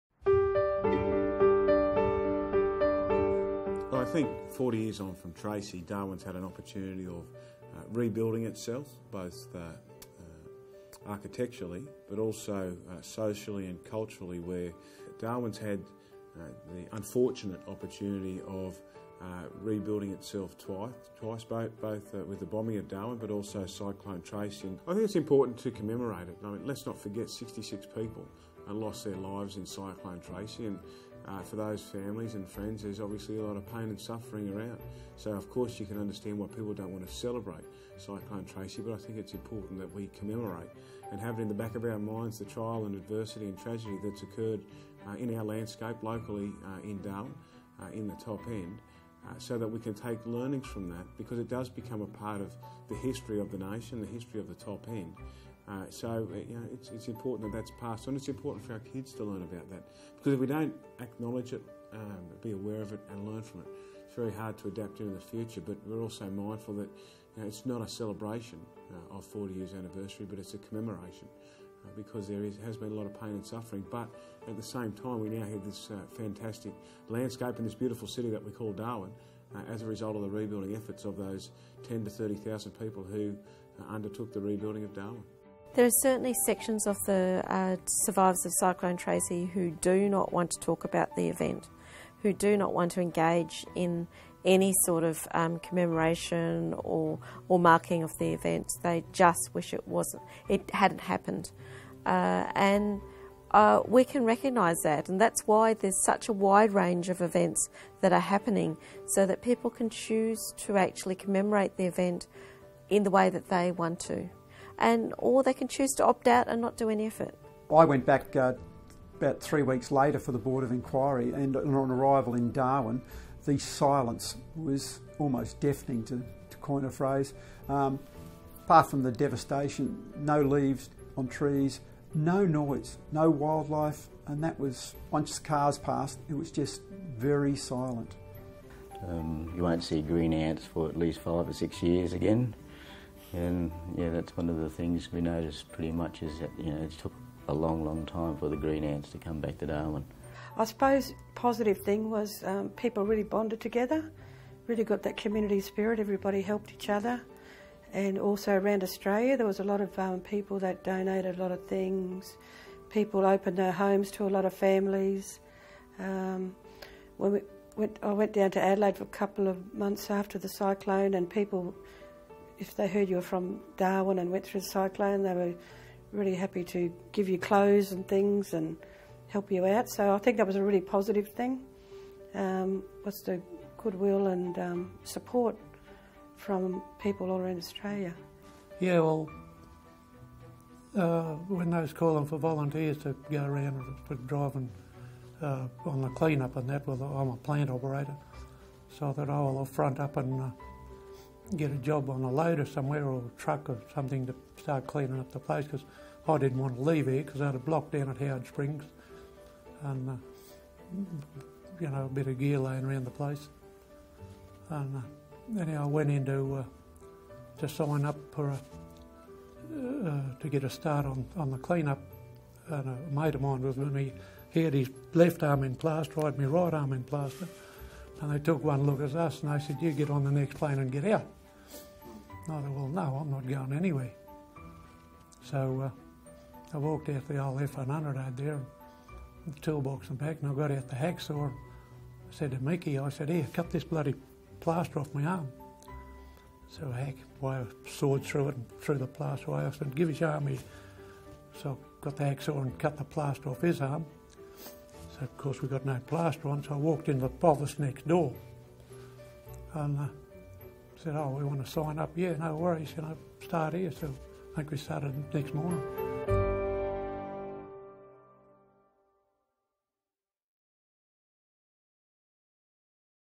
40 years on from Tracy 04:33
The city of Darwin has been rebuilt twice. Once after the bombing of Darwin and once after Cyclone Tracy.

    40 years on from Tracy
    04:33

    40 years on from Tracy
    04:33

    40 years on from Tracy
    04:33